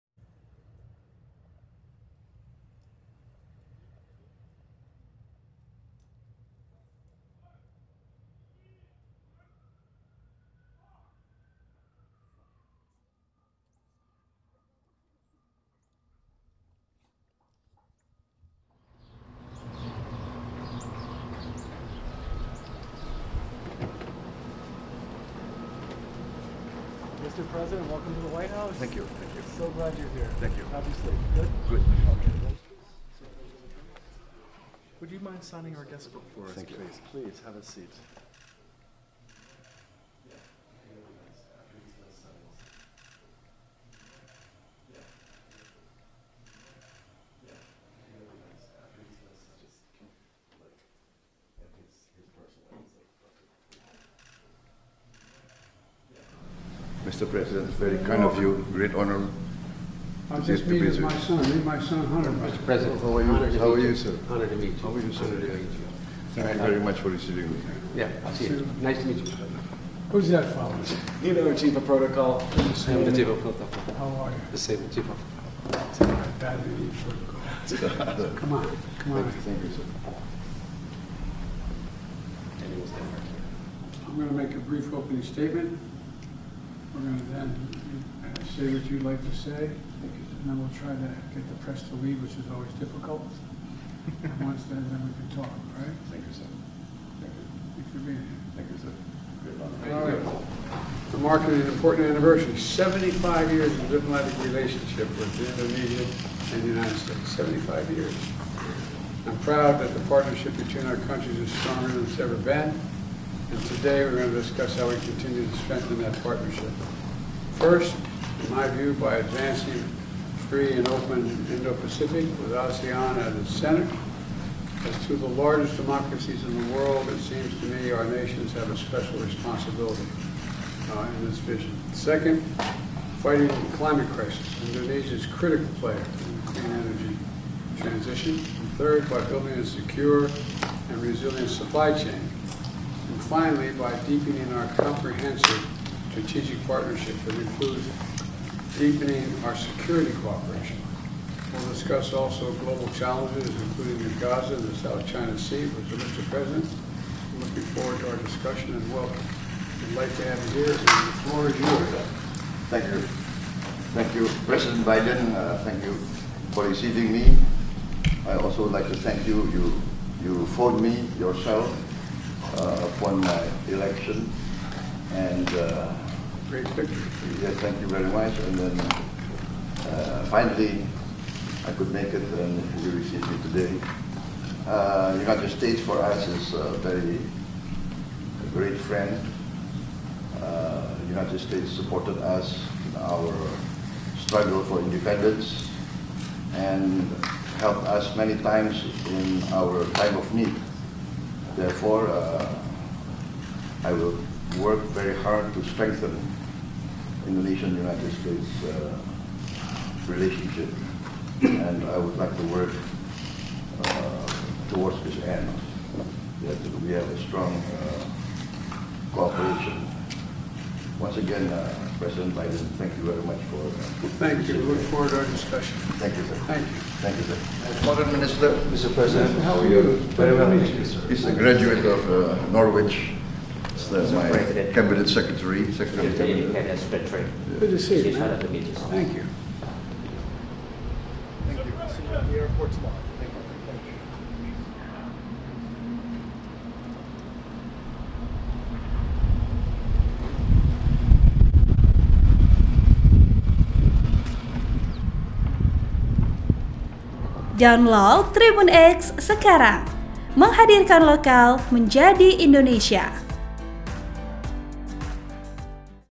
Isi Pertemuan Prabowo & Joe Biden di White House. Bahas Gaza sampai Kerja Sama Keamanan
Presiden Republik Indonesia, Prabowo Subianto, melakukan pertemuan bilateral dengan Presiden Amerika Serikat, Joe Biden, di Gedung Putih, Washington DC, pada Selasa, 12 November 2024. Pertemuan tersebut merupakan bagian dari rangkaian kunjungan kerja resmi Presiden Prabowo di Amerika Serikat guna memperkuat hubungan bilateral antara kedua negara.
Momen ini menandai formalitas awal dari pertemuan tingkat tinggi antara dua negara yang memiliki sejarah panjang dalam hubungan diplomatik.
Pertemuan ini memberikan kesempatan bagi kedua pemimpin untuk membahas sejumlah isu strategis, termasuk kerja sama di sejumlah bidang dalam suasana yang bersahabat dan penuh hormat. Turut hadir mendampingi Presiden Prabowo dalam pertemuan tersebut adalah Menteri Luar Negeri Sugiono dan Sekretaris Kabinet Teddy Indra Wijaya.